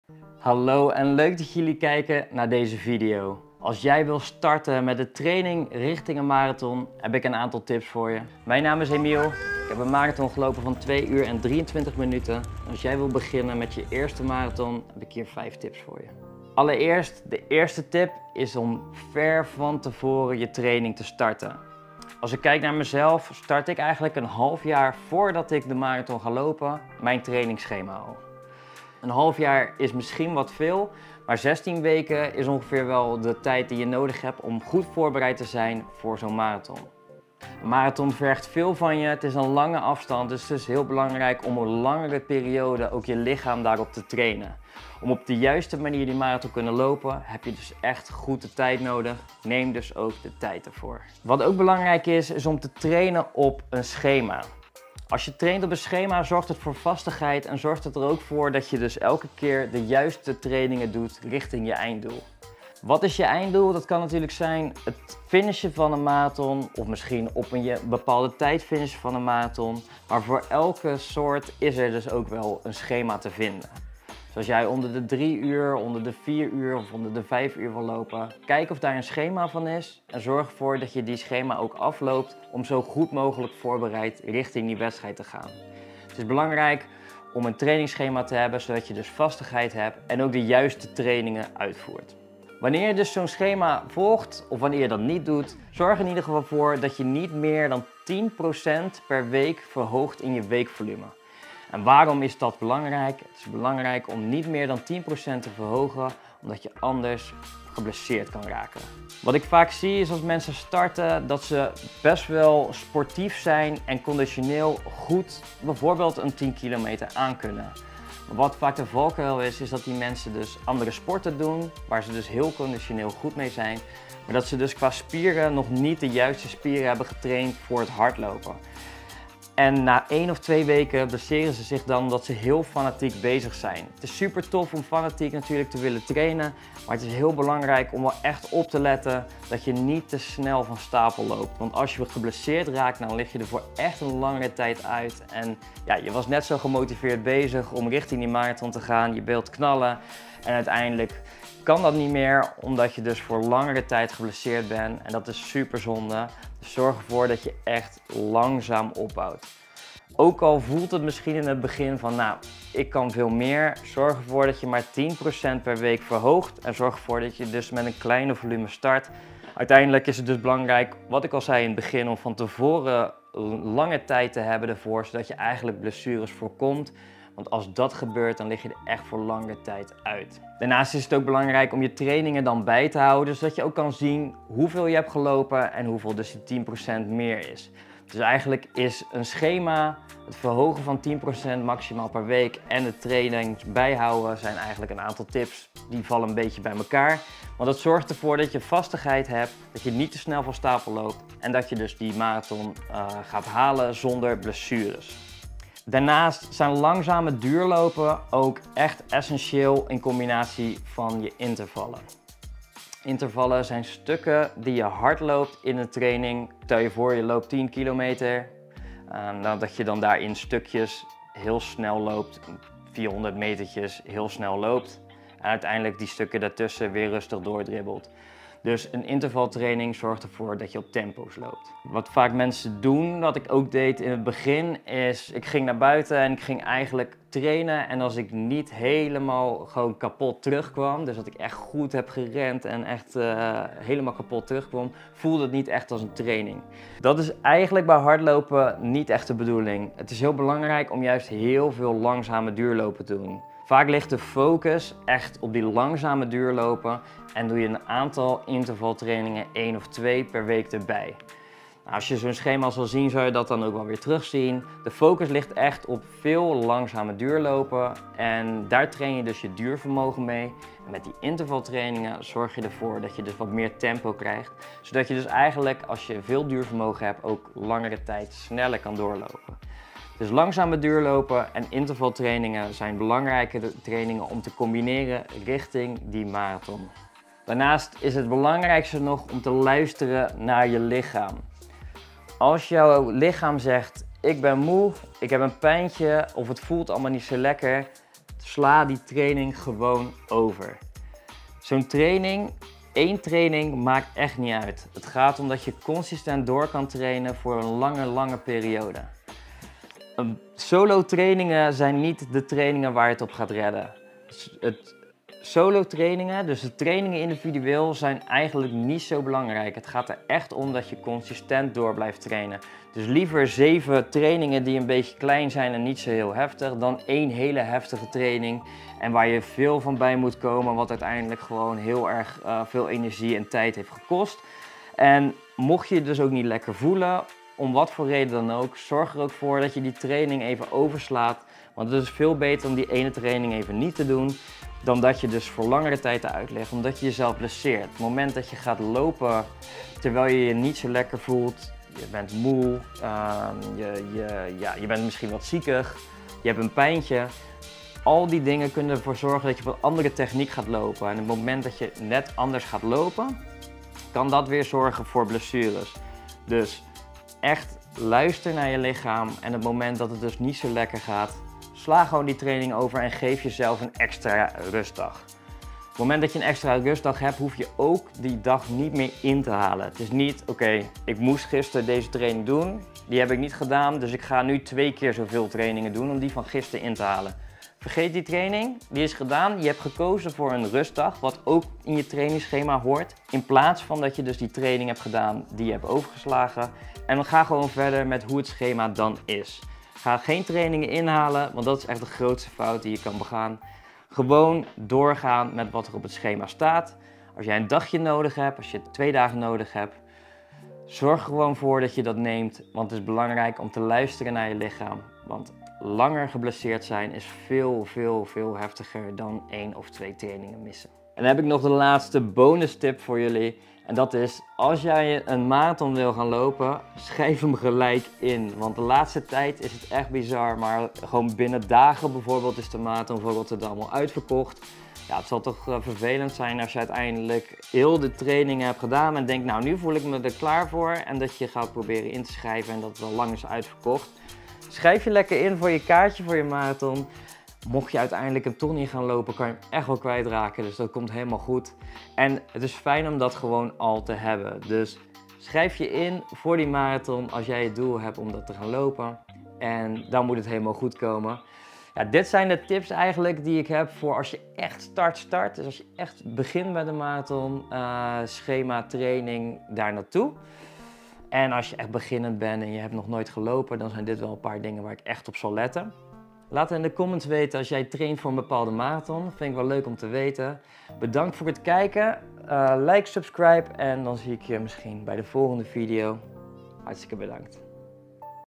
5 Tips Beginnen Marathon training! Tips voor je eerste marathon.
Hoogtestage in Kenia, 🇰🇪 YouTube serie Bedankt voor de hulp!